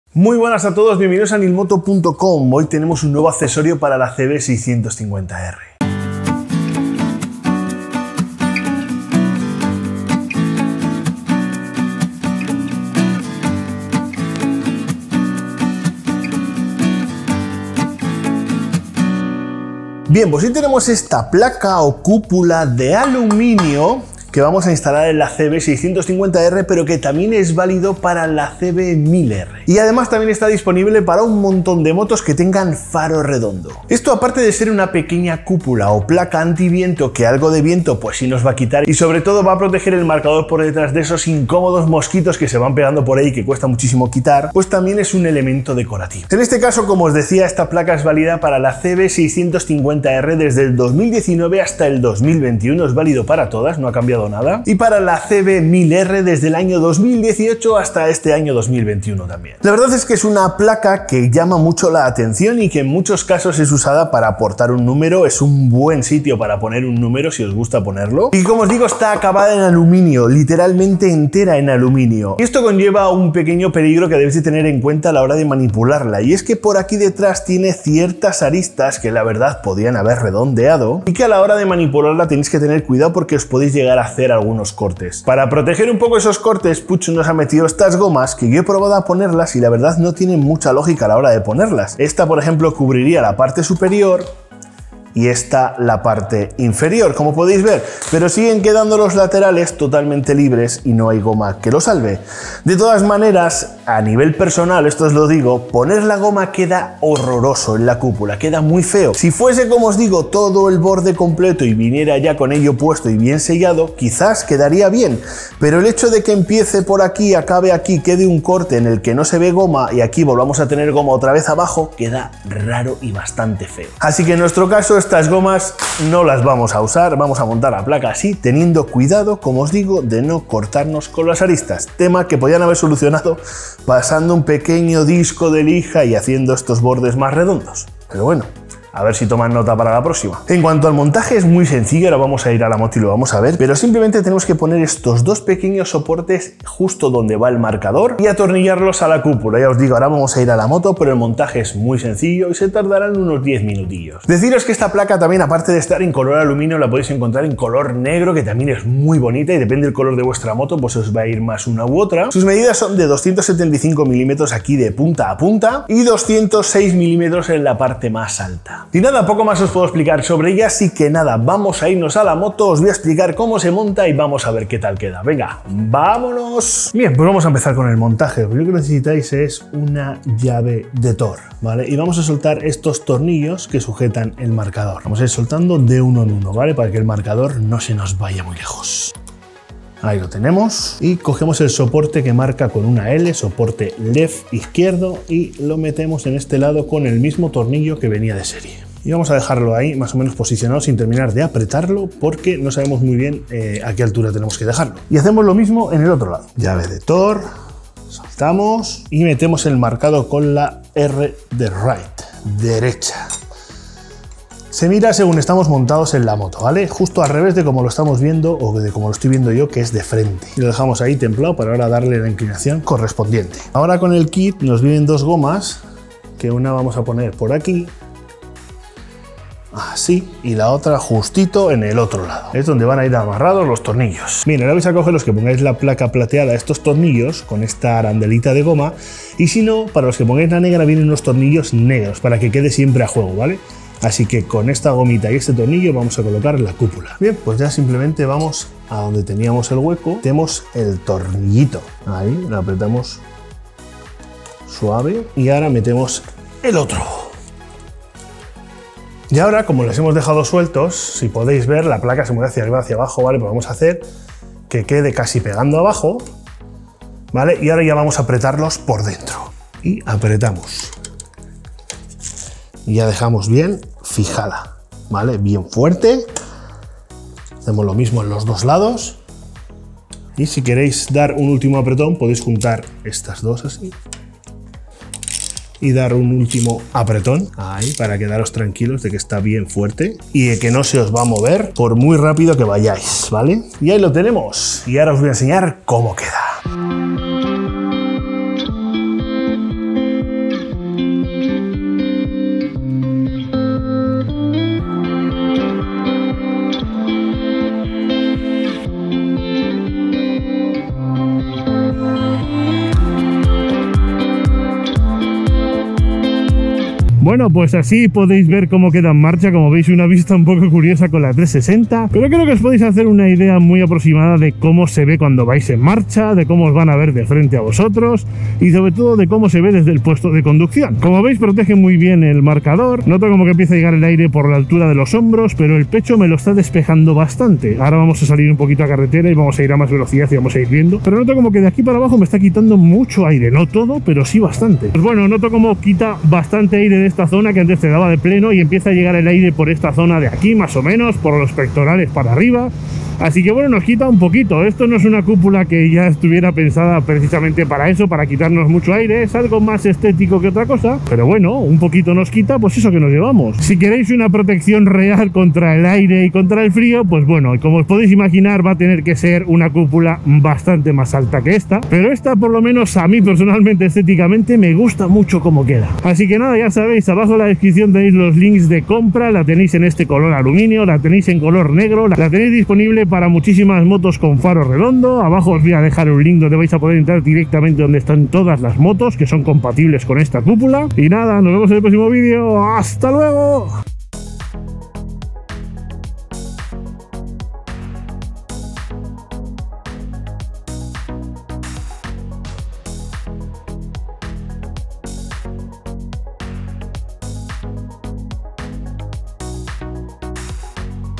😲 Es de ALUMINIO!! Cúpula/Placa porta números //Nilmoto
Hoy os traemos una cupula/placa algo atipica y muy curioso hacha en aluminio!
Hace las veces de cupula anti vientos y placa porta numeros.